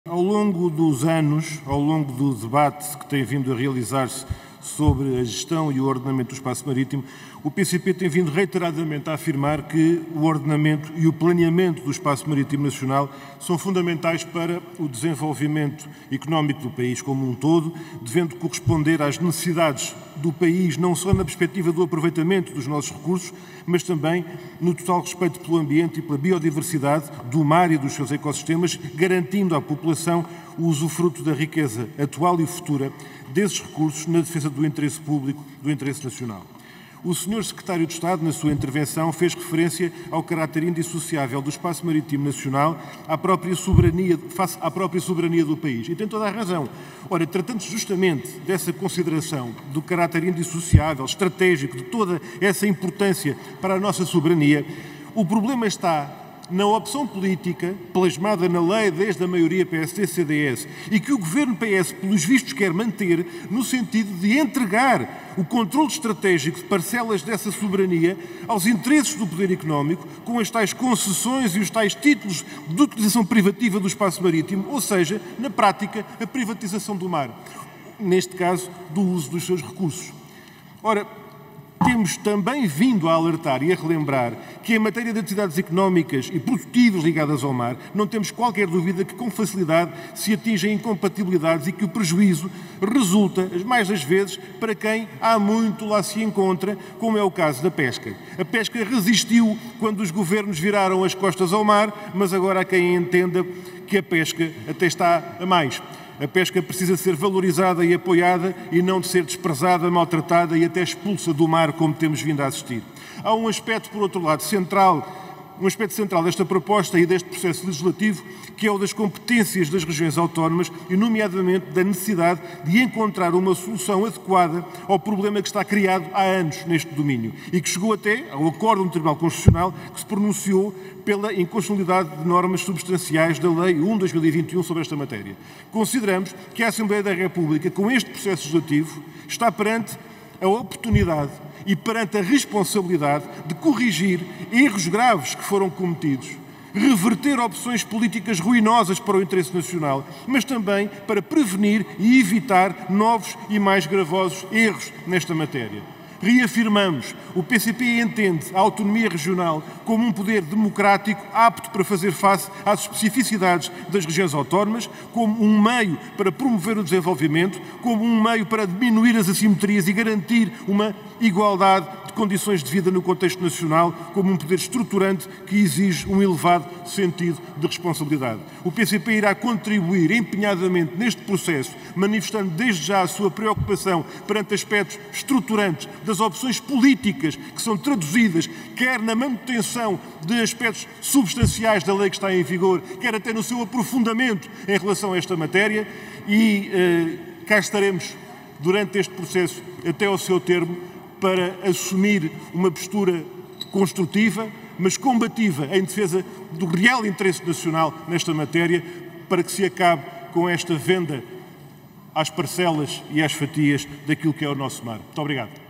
O controlo do Espaço Marítimo Português pelo Estado é fundamental para a soberania do país
Intervenção de Bruno Dias na Reunião Plenária (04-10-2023)